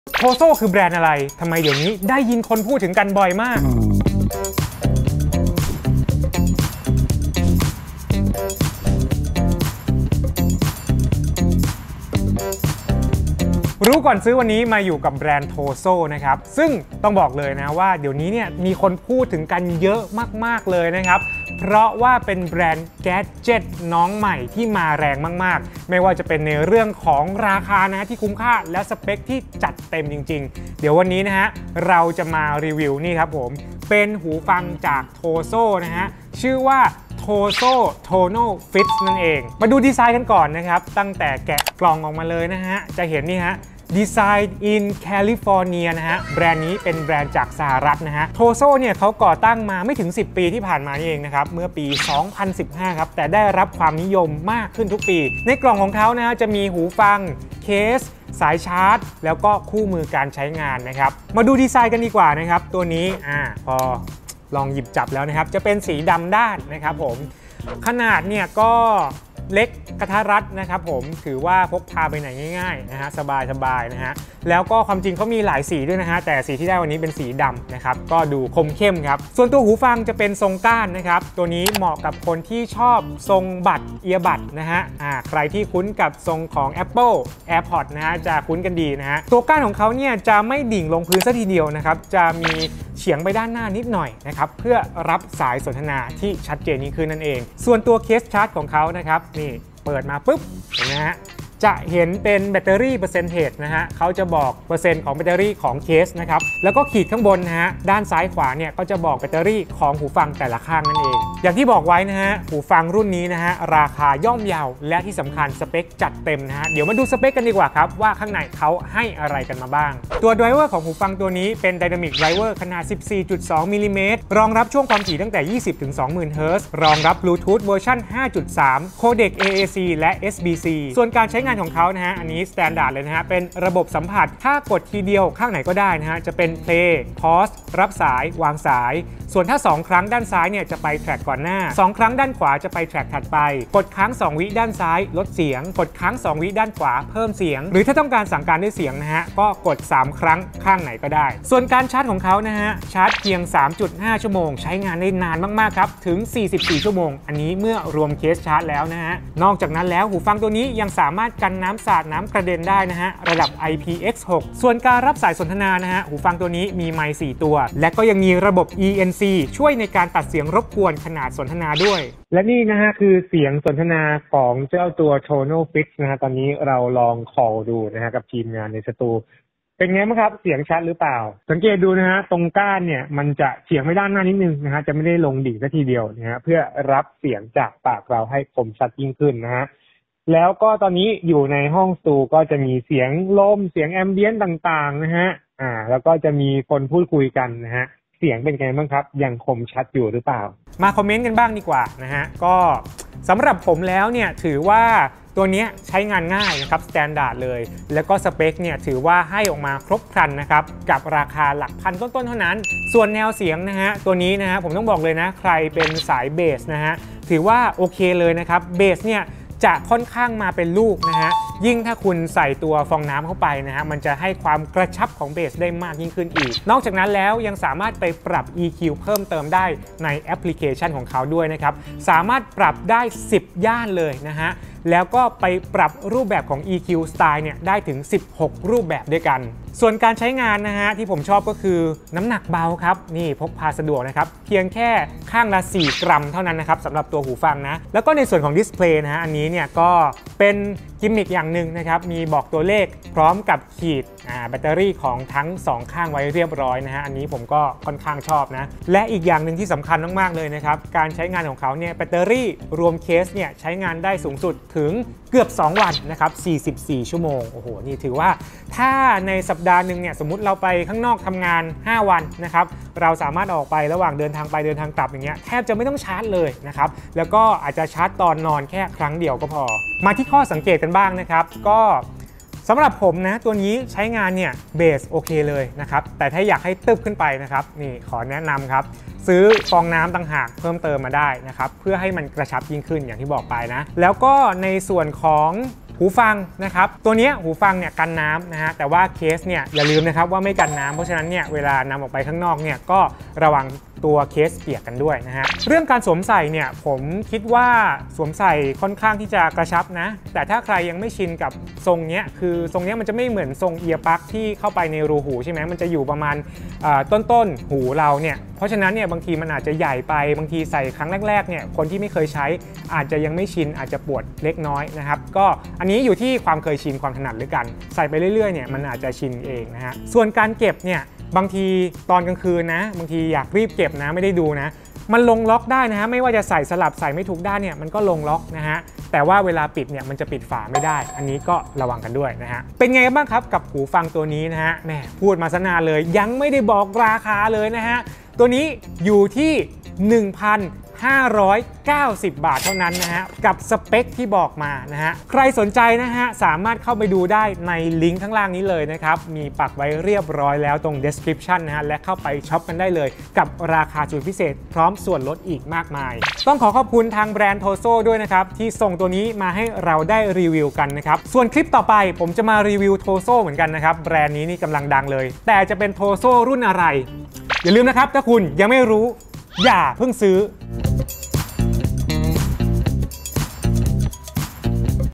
รู้ก่อนซื้อ: พันกว่าบาทจะเด็ดแค่ไหน??? TOZO Tonal Fits หูฟังที่กำลังมาแรงในราคาน่าคบ!
รู้ก่อนซื้อ: TOZO Tonal Fits พันนิด ๆ แต่สเปกไม่ธรรมดา!
ติดตามข้อมูลข่าวสาร 𝗧𝗢𝗭𝗢  ได้ที่

𝗧𝗢𝗭𝗢 𝗧𝗼𝗻𝗮𝗹 𝗙𝗶𝘁𝘀
ราคา 1,590 บาท ประกันศูนย์ไทย 1 ปี

0:00-0:39 intro
0:39-1:51 ดีไซน์
1:52-2:51 สเปกและการใช้งาน
2:52-3:21 ทดสอบเสียงไมค์
3:21-5:43 คอมเมนต์
5:45-6:11 ราคา